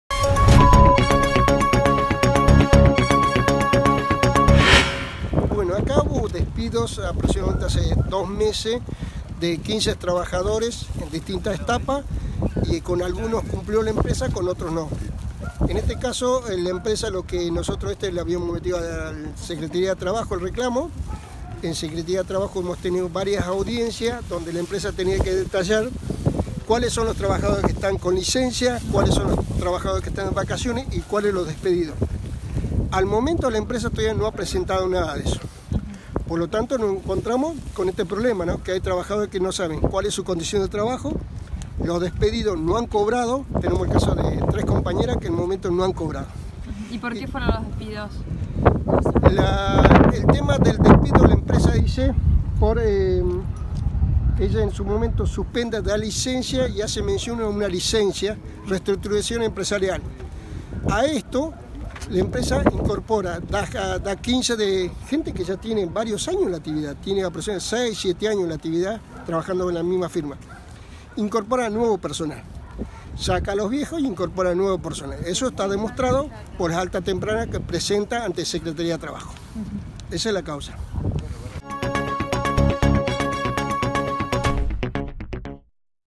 TRABAJADORES DE LA PESCA RECLAMAN CONTRA DESPIDOS SIN INDEMNIZACIÓN
Alfredo Viltes/Sindicato de la Alimentación

EnteratePrimero - Canal de Noticias de Comodoro Rivadavia y Chubut.